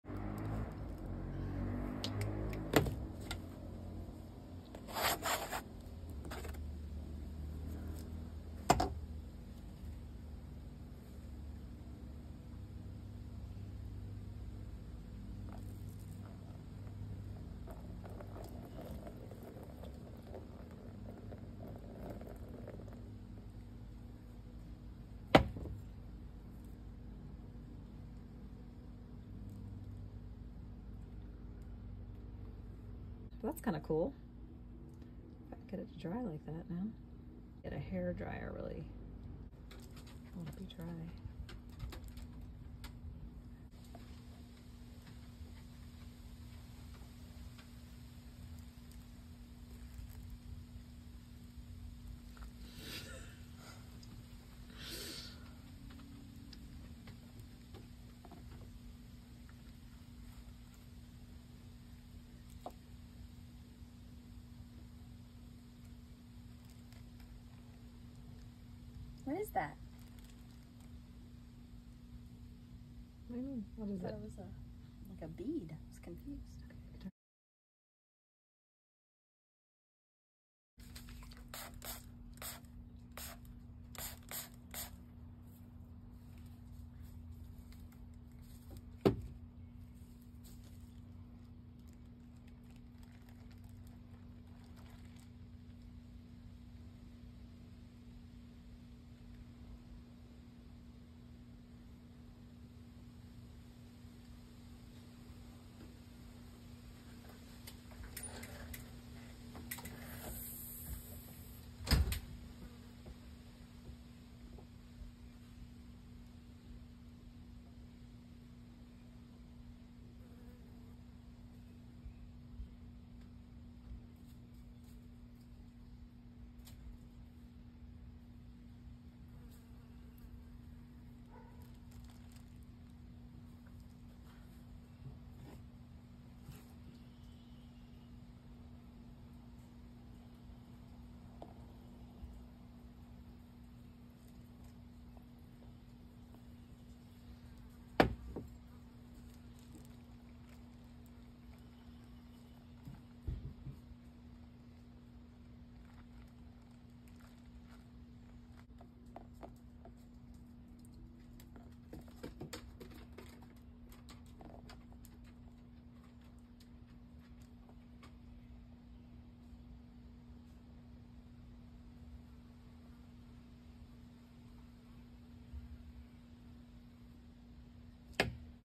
ASMR Painting with Coffee, Acrylic & Ink
Hi Everyone 👀👂🏼☕️🎨
So glad you’re here! I hope you enjoy me trying out new stuff!
Today I’m using coffee (needs to be much stronger next time), Acrylic Paint and Acrylic Ink.
I just love the ASMR effect, paint just SOUNDS fun!